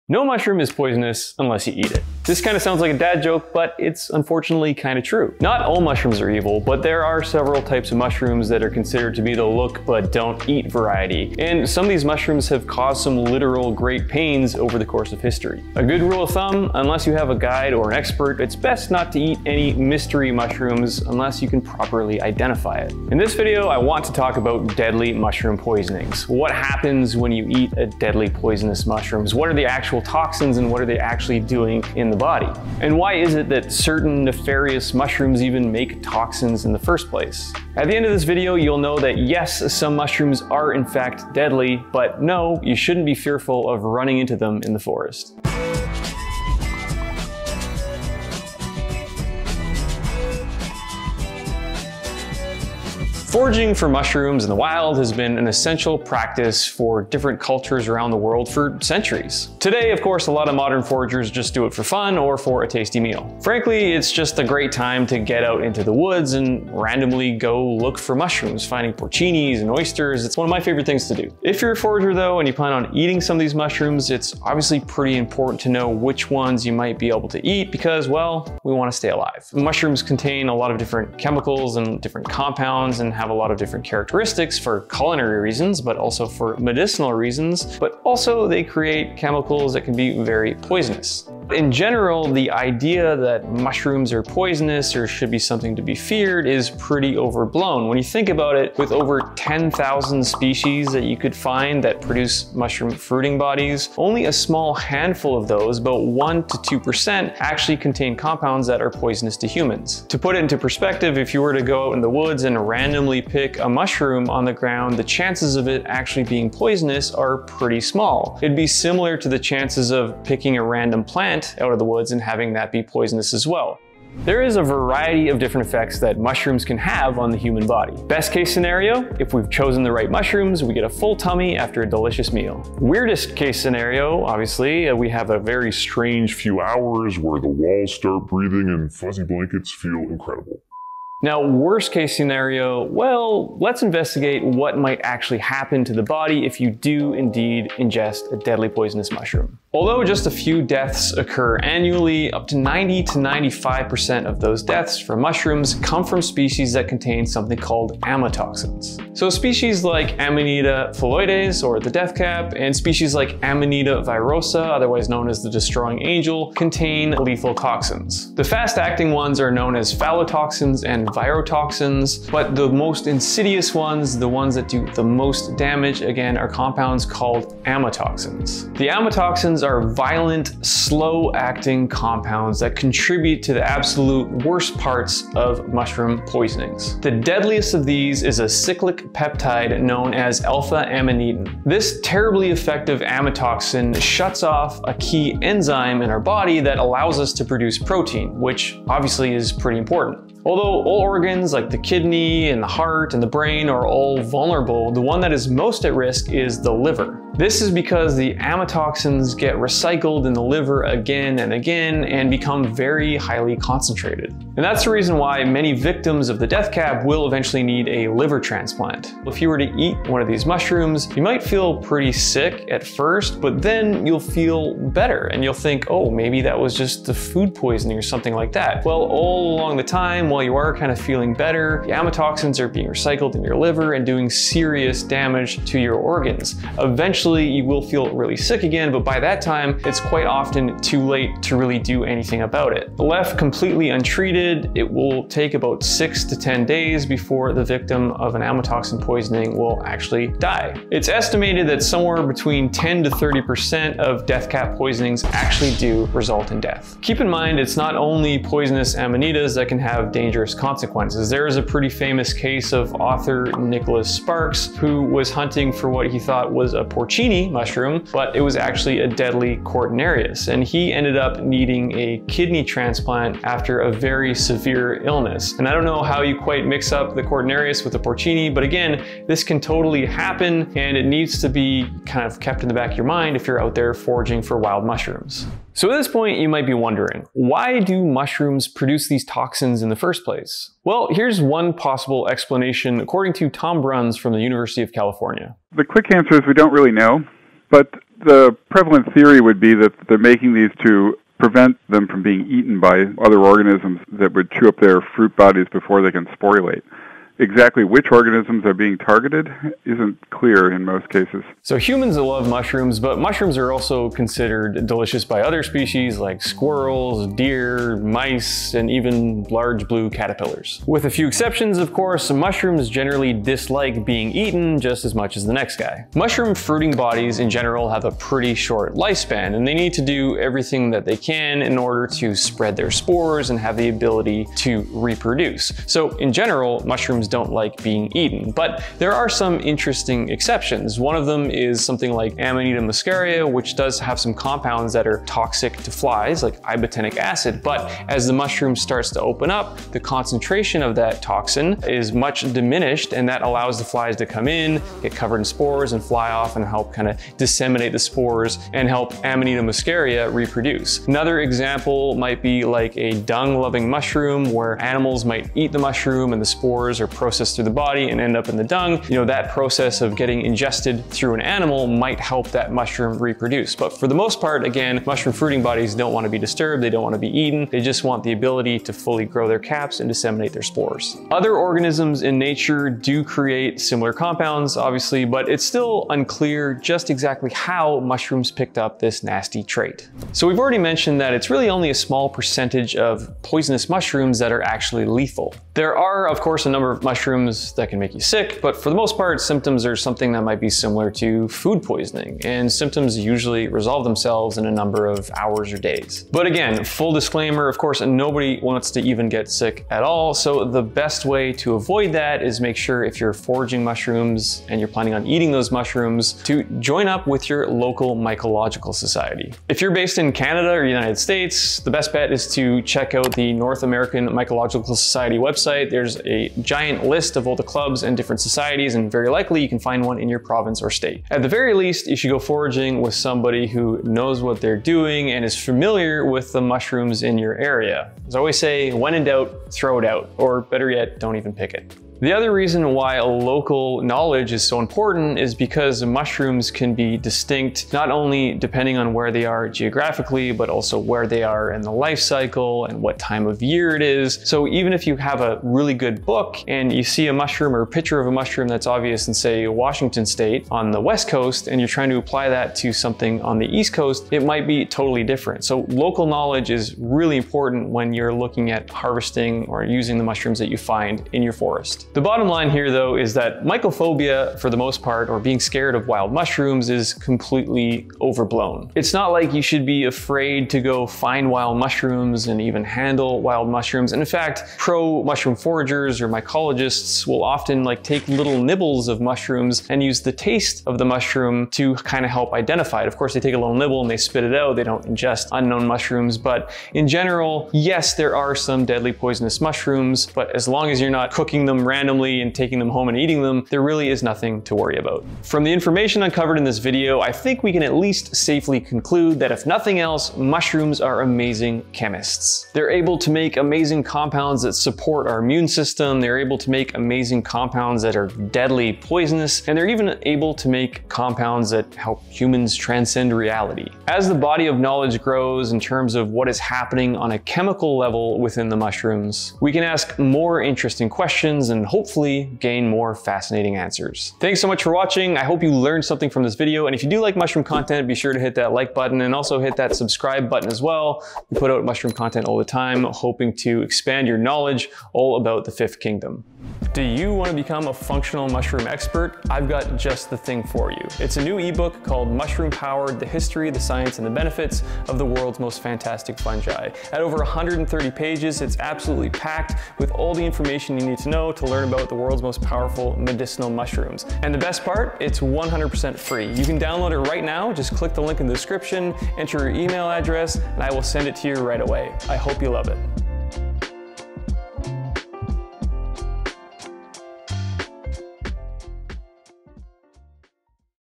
☠️ DEADLY Mushrooms (Should You Be Scared of Harvesting Wild Mushrooms?)
What happens when you eat a poisonous mushroom? In this video, we go over the basics of the "deadly" mushrooms, what toxins they contain, and tips to avoid accidentally ingesting one of these dangerous shrooms.

0:00 Intro
0:53 How many mushrooms are actually poisonous?
2:30 What happens when you eat a deadly poisonous mushroom?
4:46 Why do mushrooms produce toxins?
6:30 How to avoid poisonous mushrooms
8:26 Outro

🎉 The FreshCap team is launching our enormous mushroom E-Book!! Get yours for FREE by signing up here: